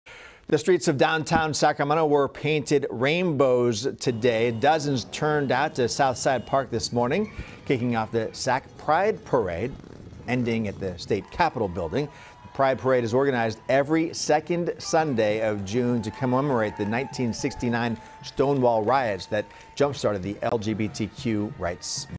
Pride March takes streets of dowtown Sacramento
Crowds turned out to Southside Park on Sunday morning to kick off the parade.